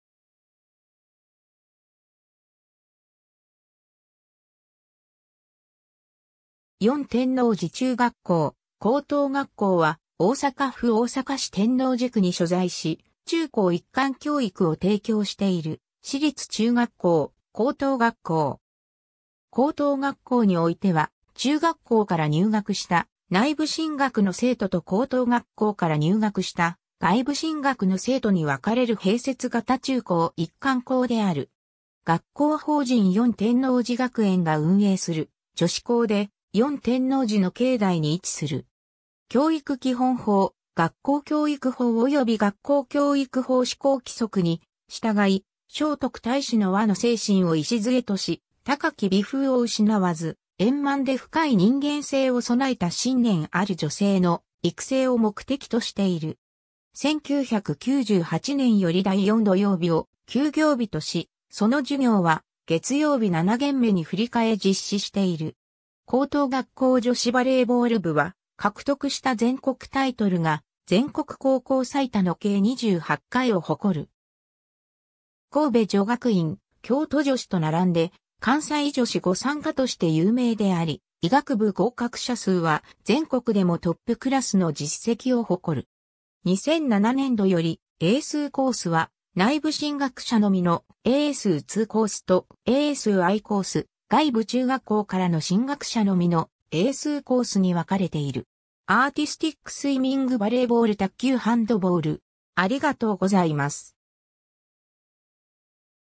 四天王寺中学校・高等学校
四天王寺中学校・高等学校（してんのうじちゅうがっこう・こうとうがっこう）は、大阪府大阪市天王寺区に所在し、中高一貫教育を提供している私立中学校・高等学校。
 高等学校においては、中学校から入学した内部進学の生徒と高等学校から入学した外部進学の生徒に分かれる併設型中高一貫校（別クラス型）である。
 学校法人四天王寺学園が運営する女子校で、四天王寺の境内に位置する。
 教育基本法、学校教育法および学校教育法施行規則に従い、聖徳太子の和の精神を礎とし、高き美風を失わず、円満で深い人間性を具えた信念ある女性の育成を目的としている。
 1998年より第4土曜日を休業日とし、その授業は月曜日7限目に振り替え実施している。
 高等学校女子バレーボール部は、獲得した全国タイトルが全国高校最多の計28回（選手権11回、国体13回、選抜4回）を誇る。
（9人制を含む） 神戸女学院、京都女子と並んで関西女子御三家として有名であり、医学部合格者数は全国でもトップクラスの実績を誇る。
 （スポーツ・芸術コースの生徒しか参加できない部活動がある。） ※2007年度より、英数コースは、内部進学者のみの「英数IIコース」と「英数Iコース」、外部中学校からの進学者のみの「英数コース」に分かれている。
 アーティスティックスイミング バレーボール 卓球 ハンドボール